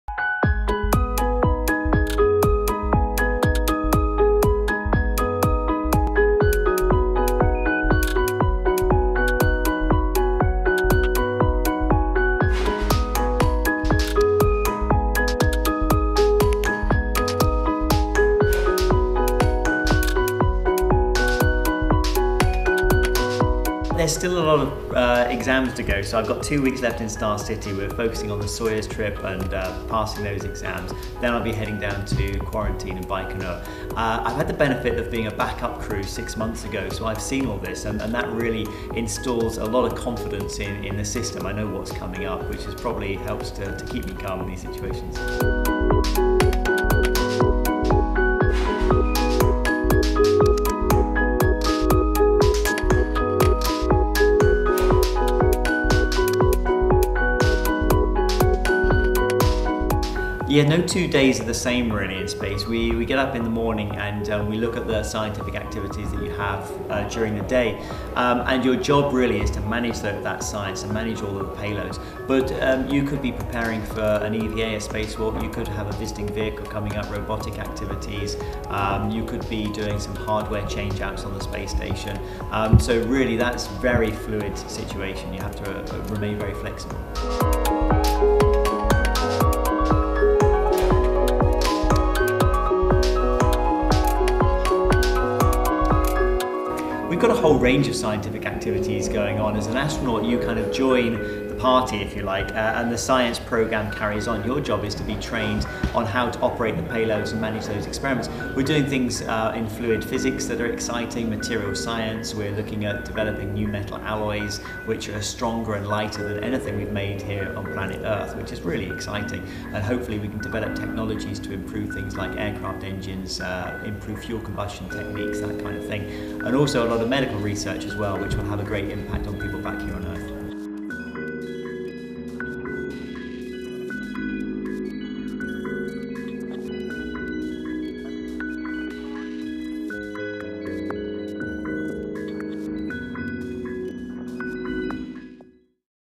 Meet Timothy Peake, the astronaut boldly going where no Brit has gone before
Timothy Peake is set to become the first British astronaut to visit the International Space Station.